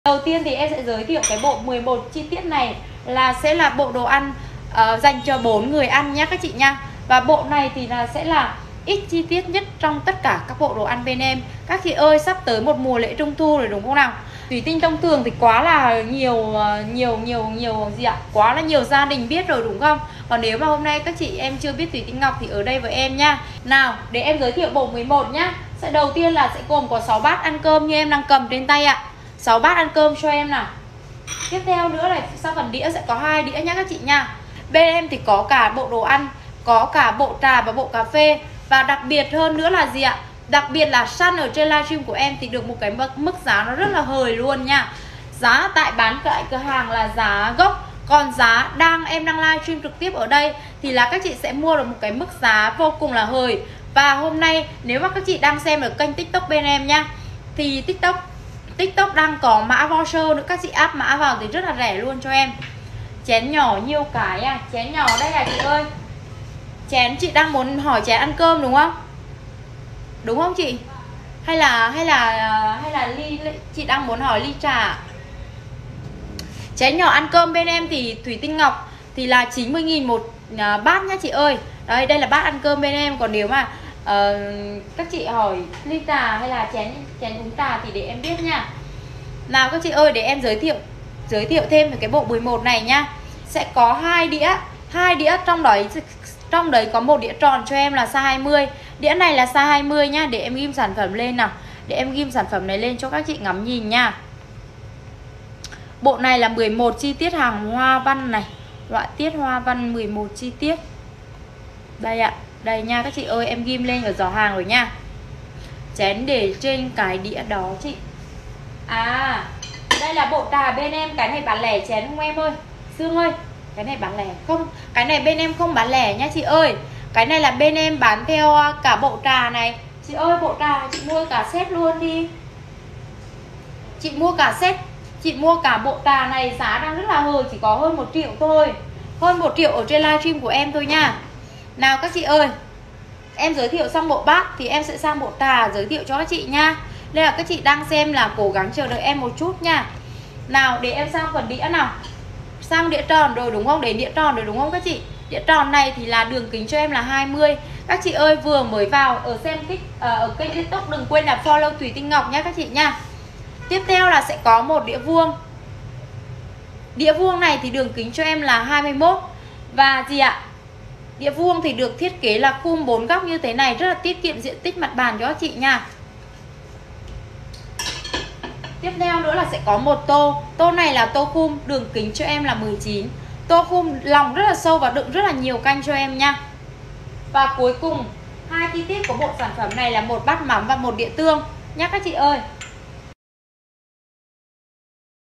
Bộ bát đĩa 11 chi tiết Thủy Tinh Ngọc dành cho gia đình 4 người khai cỗ mùa trung thu
Thủy Tinh Ngọc – sự lựa chọn hoàn hảo cho gia đình bạn!